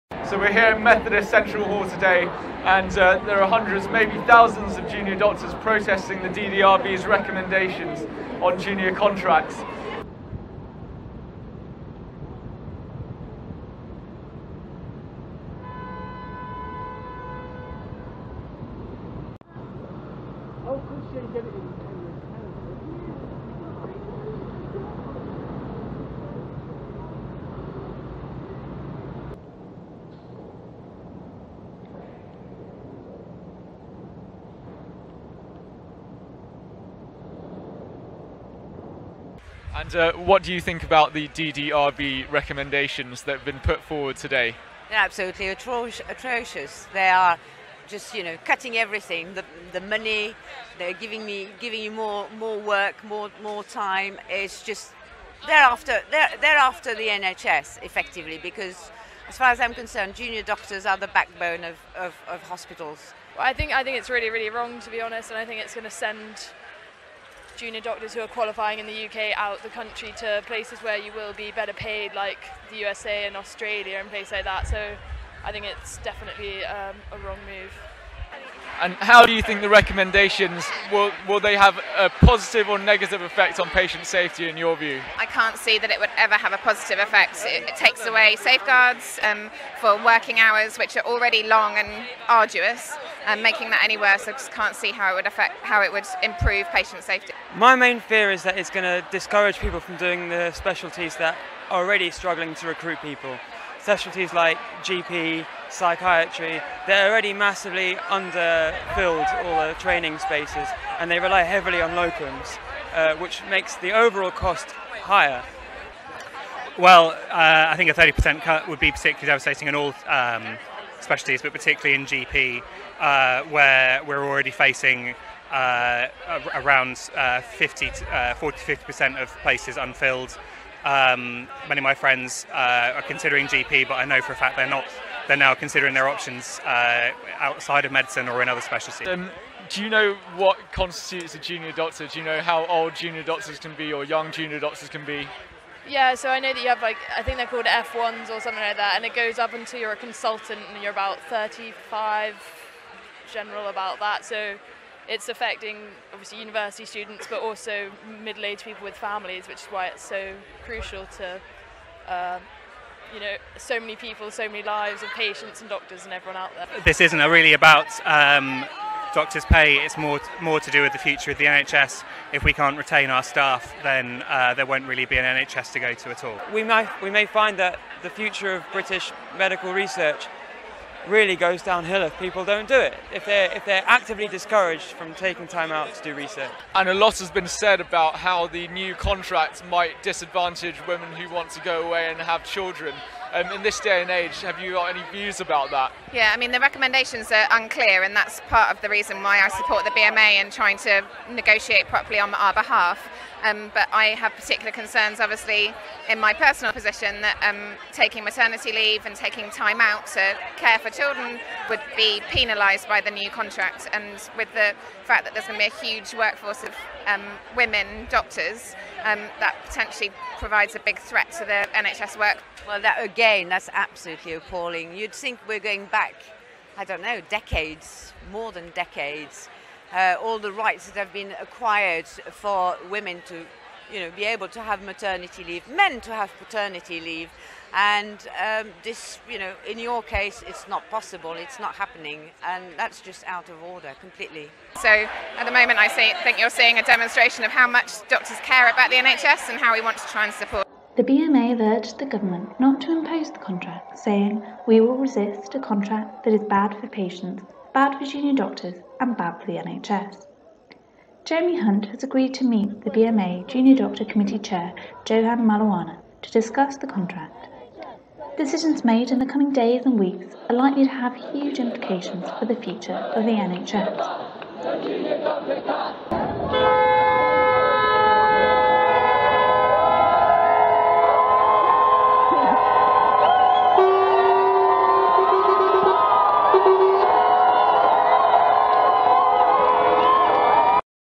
Junior Doctors Protest! What's the word on the street
On 28th September 2015, 5000 NHS junior doctors walked the streets of Westminster, London, Uk to protest a new contract that would be 'imposed' on them by the Department of Health.

Whilst the government claimed that the new contracts would be fairer for both patients and staff, the British Medical Association labelled them unsafe.  A lot has been said by both sides through official channels - but staff and patients of the NHS have been relatively unheard in a battle between government and union.

We reached out to those protesting, and onlooking members of the public to find out what people really think.